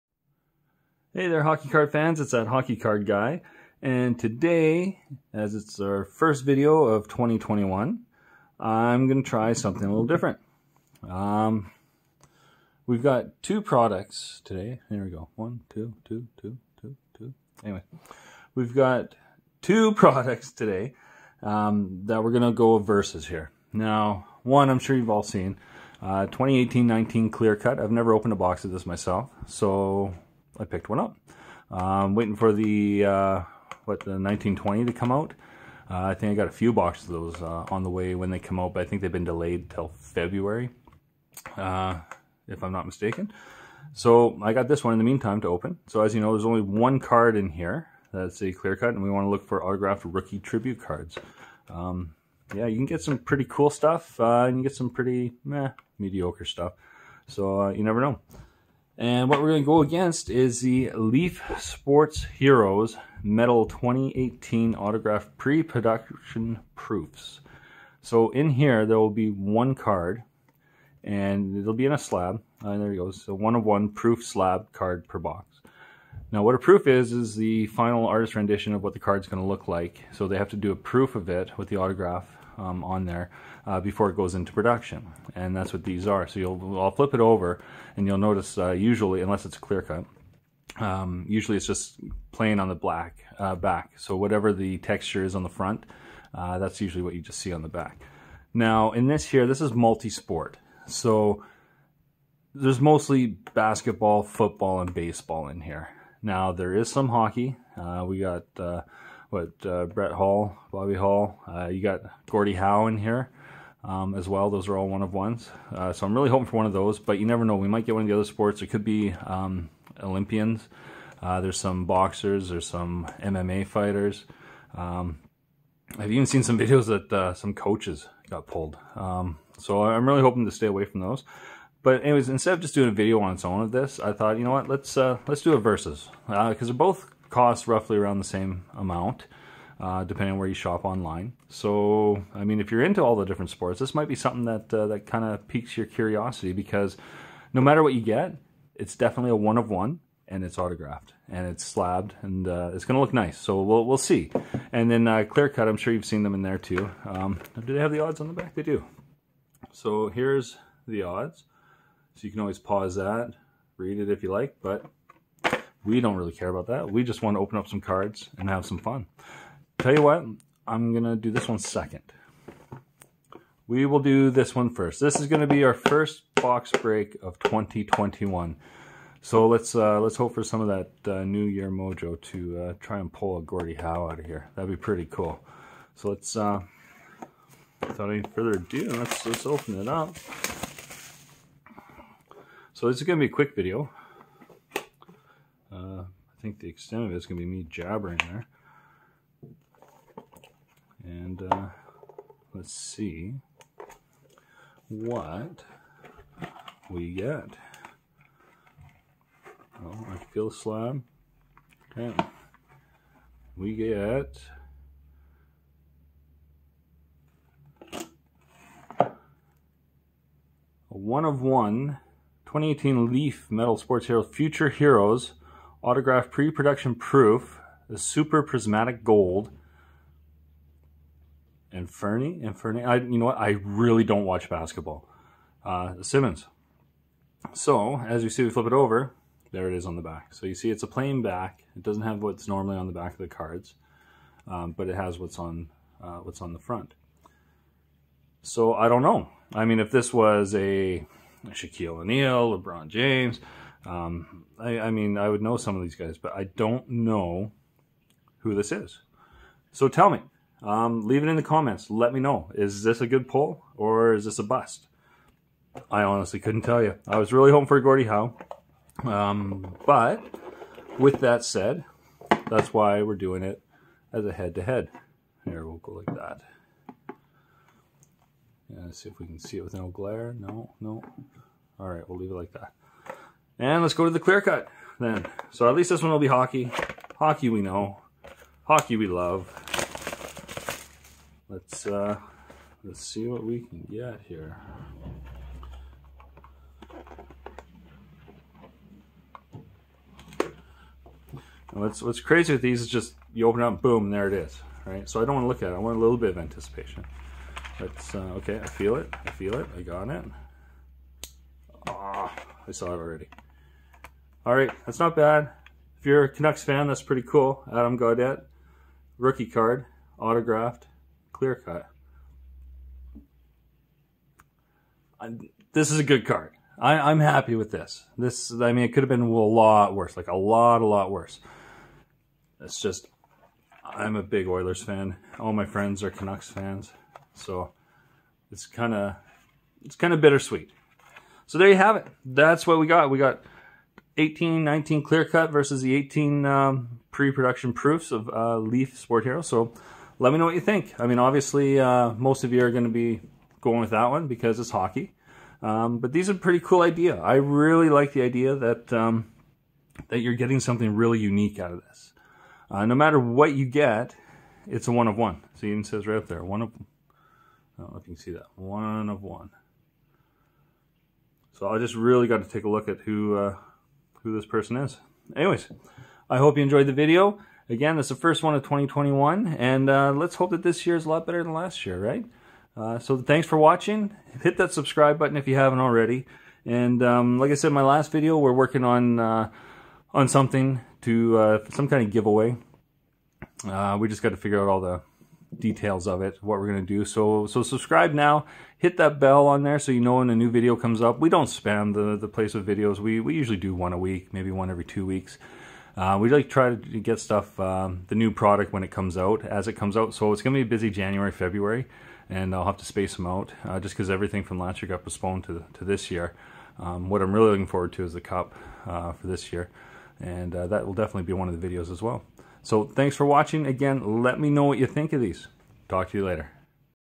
18-19 Clear Cut vs 2018 Leaf Metal Sports Heros Proofs
Happy New Years Everyone, lets have a great year full of hits! I thought I'd try something different for the first break of the year and try our luck at a multisport product so I figured I'd put it up against another 1of1 product in Clear Cut, just to be sure we got some hockey. Let me know what product you like better. Cheers

Cardboard Connection has the best card series info out there. Here's a link to these sets info: